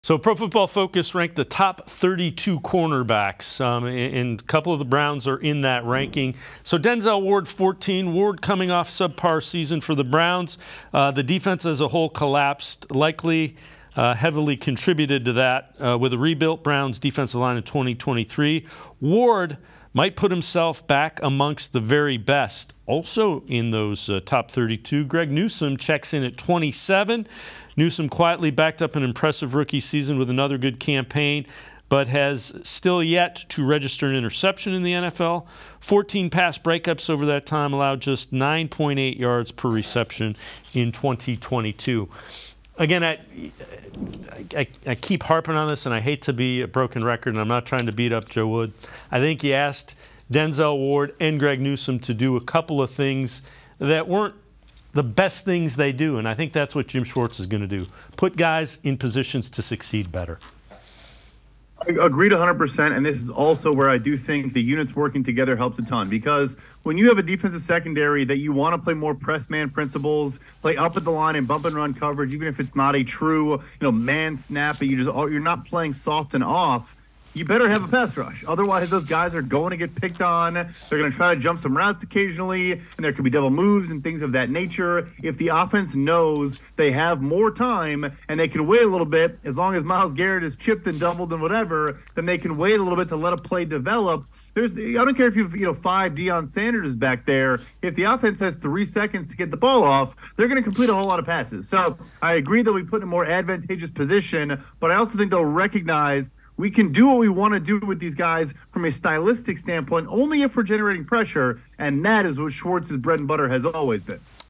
Why Denzel Ward & Greg Newsome Will Be Better With the Browns in 2023 - Sports4CLE, 5/31/23
Dave Bacon and Brad Spielberger discuss Pro Football Focus' cornerback rankings and where Denzel Ward and Greg Newsome fall on the list.